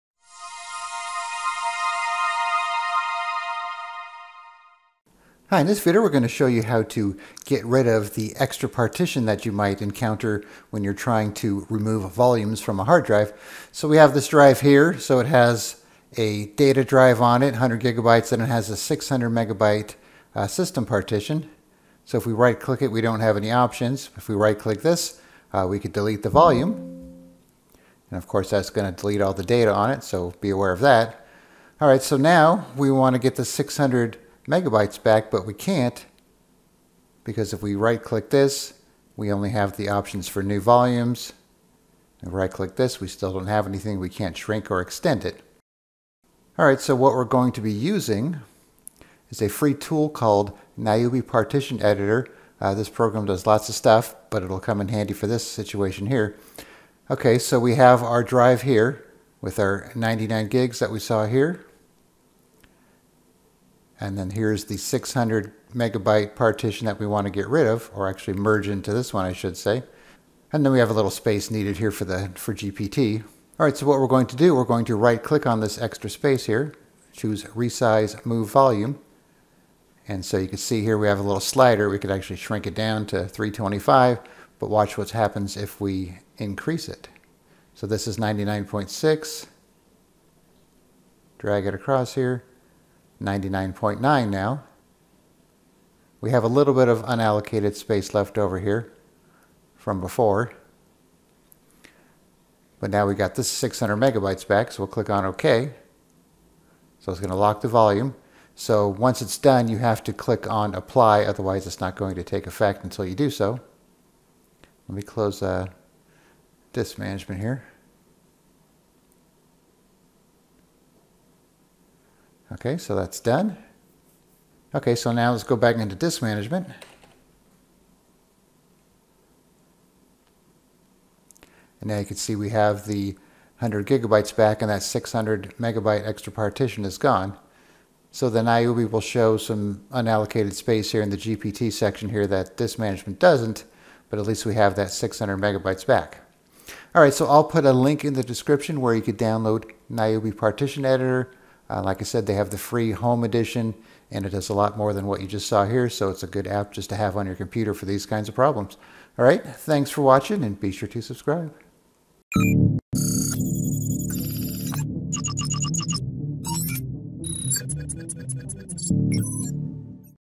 How to Remove or Merge Extra Volumes from Your Hard Drive that Disk Management Can't Delete
If you have a volume on a hard drive such as an EFI system partition that you cannot delete or merge into another volume, then you might find yourself giving up valuable disk space on your drive. When this happens, you will find that you cannot delete, shrink or expand the leftover volume from the Windows Disk Managment tool. Fortunately, there is another free app you can use to fix this problem.

Here is a link to download the free NIUBI Partition Manager software.